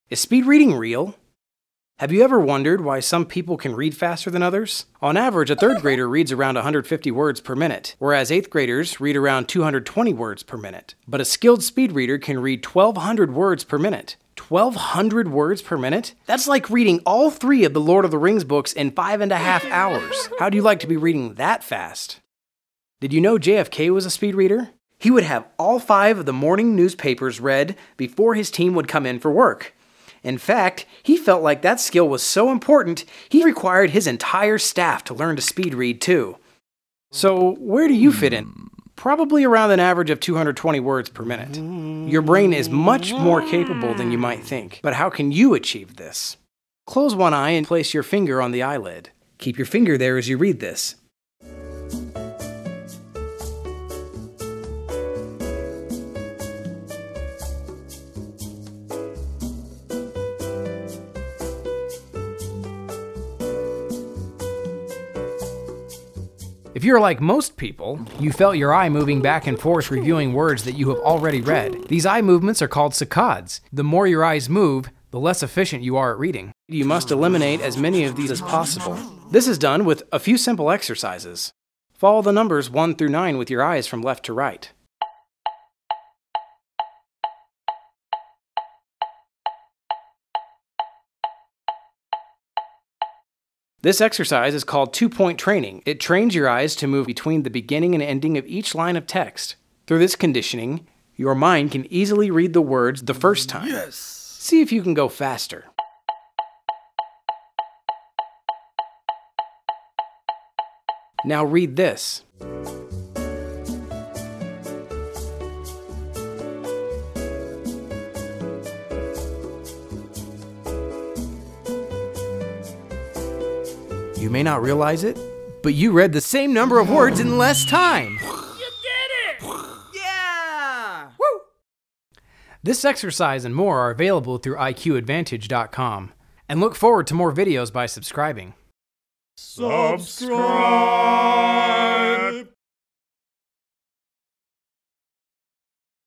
Is Speed Reading Real?
Many people wonder if speed reading is real, or if it's a hoax. We answer this question by showing you how you can increase your speed with only a few seconds of training!